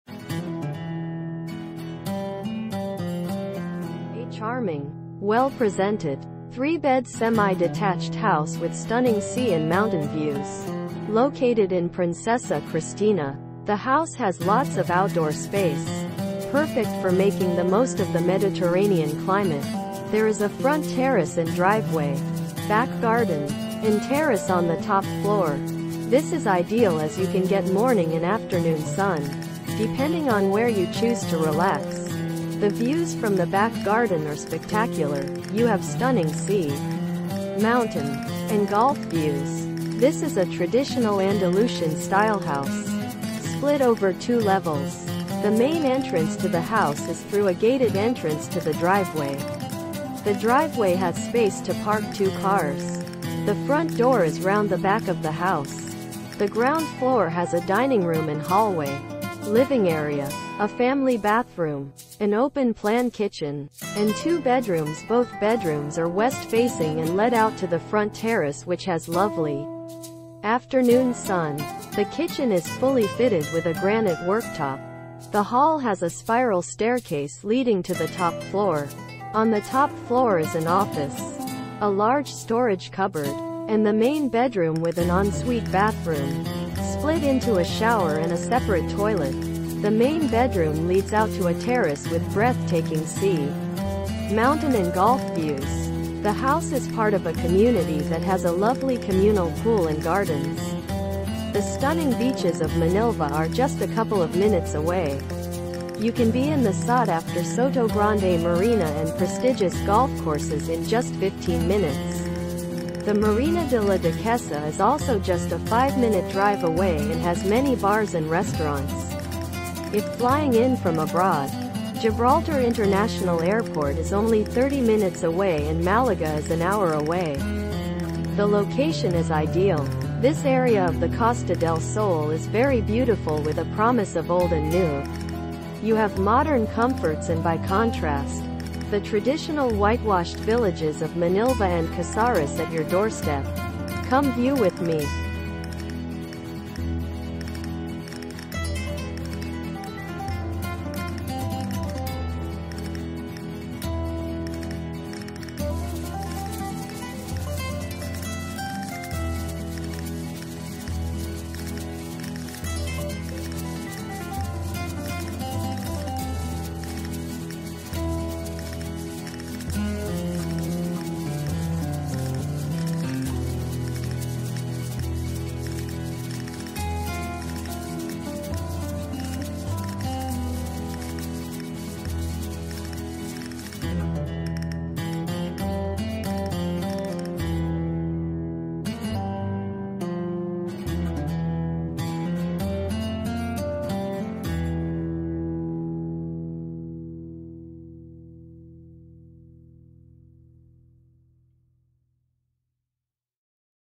Semi-Detached House in Manilva, Costa Del Sol FOR SALE €269000
Semi-Detached House in Manilva, FOR SALE €269000 Property ID: CDS19703
 A charming, well-presented, three-bed semi-detached house with stunning sea and mountain views, located in Princesa Kristina.  The house has lots of outdoor space, perfect for making the most of the Mediterranean climate.  There is a front terrace and driveway, back garden, and terrace on the top floor.  This is ideal as you can get morning and afternoon sun, depending on where you choose to relax.  The views from the back garden are spectacular - you have stunning sea, mountain, and golf views.
This is a traditional Andalucian-style house, split over two levels.  The main entrance to the house is through a gated entrance to the driveway.  The driveway has space to park two cars.  The front door is round the back of the house.
The ground floor has a dining room and hallway, living area, a family bathroom, an open-plan kitchen, and two bedrooms.  Both bedrooms are west facing and lead out to the front terrace which has lovely afternoon sun.  The kitchen is fully fitted with a granite worktop.  The hall has a spiral staircase leading to the top floor.  On the top floor is an office, a large storage cupboard, and the main bedroom with an ensuite bathroom ( split into a shower and a separate toilet).  The main bedroom leads out to a terrace with breath-taking sea, mountain and golf views.
The house is part of a community that has a lovely communal pool and gardens.  The stunning beaches of Manilva are just a couple of minutes away.  You can be in the sought-after Sotogrande Marina and prestigious golf courses in just 15 minutes.  The Marina de la Duquesa is also just a 5-minute drive away and has many bars and restaurants.  If flying in from abroad, Gibraltar International Airport is only 30 minutes away and Málaga is an hour away.
The location is ideal.  This area of the Costa del Sol is very beautiful with a promise of old and new.  You have modern comforts and by contrast, the traditional whitewashed villages of Manilva and Casares at your doorstep.
Come view with me!
Private Terrace,Ensuite BathroomClose To Golf,Close To Port,Close To Sea,Close To Town,Close To Schools